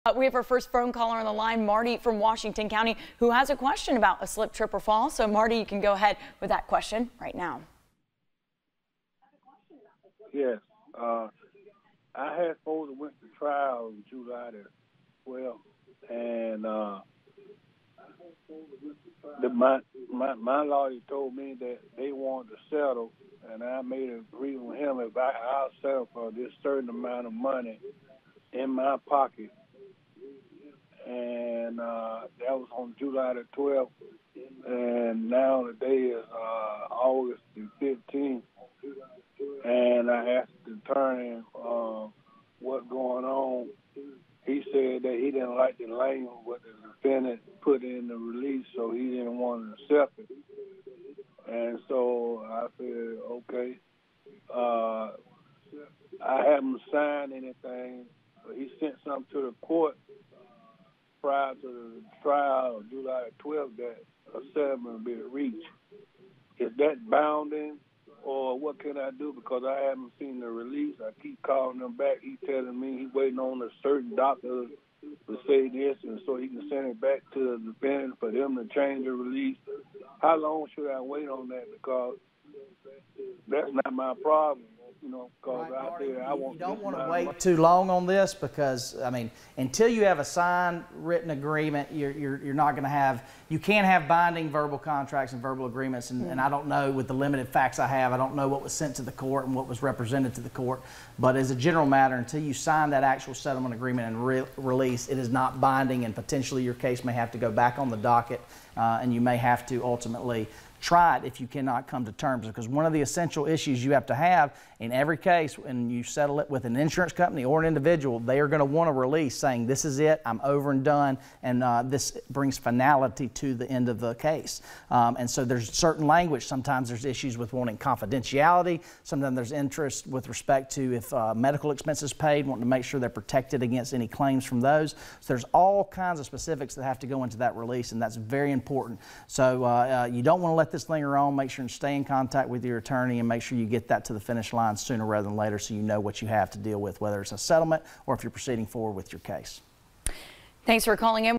LawCall WMBB Panama City 8-15-21 clip1
Attorney Ted Howell of Perry & Young, P.A. discusses the process of settling your slip & fall case on lawcall.